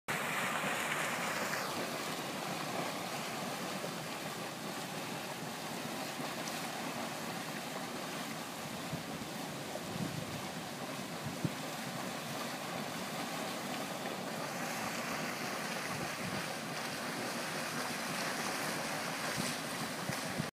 Rincón de La Vieja Volcano sulphur boiling lagoon
Sulphur smell lagoon at Rincón de La Vieja Volcano in Guanacaste !